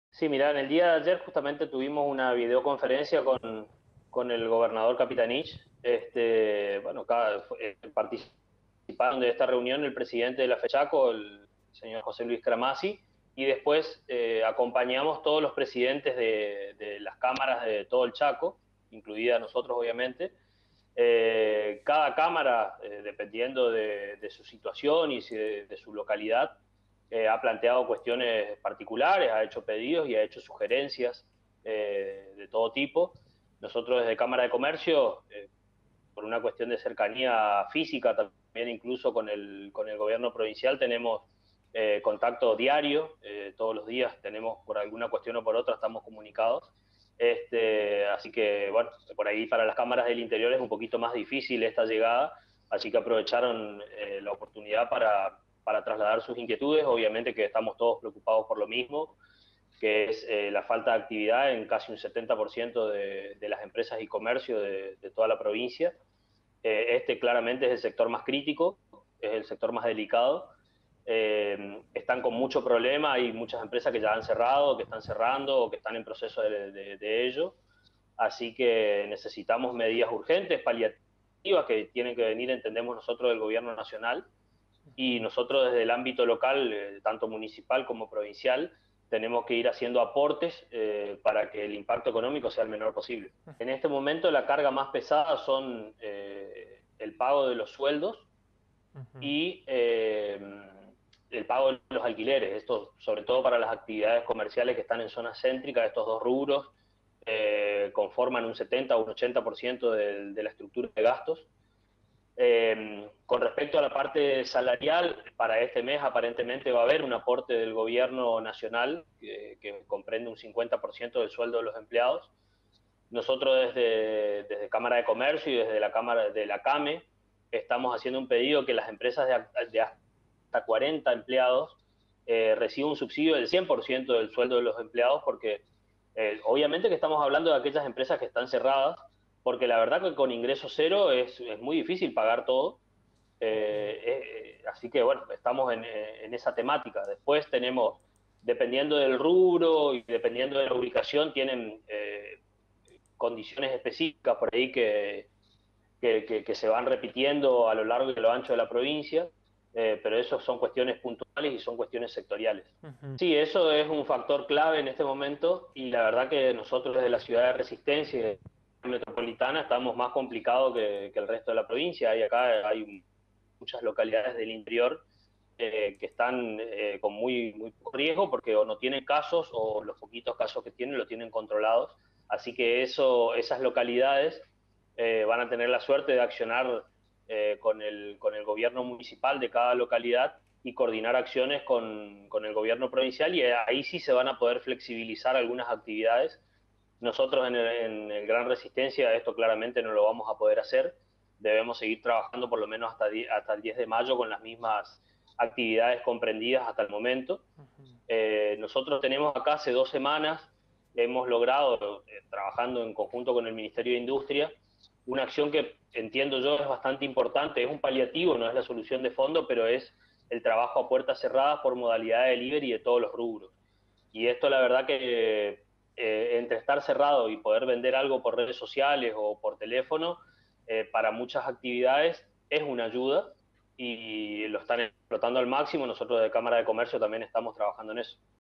GIMENEZ MARTIN- PTE CÁMARA DE COMERCIO
En virtud de la preocupación que genera la extensión de la cuarentena por el Covid-19 para el sector comercial, industrial y de producción de la provincia, dirigentes de Federación Económica del Chaco mantuvieron este lunes una reunión con el gobernador Jorge Capitanich través de una videollamada, para delinear acciones conjuntas.
Martín Giménez, Pte de la Cámara de Comercio de Resistencia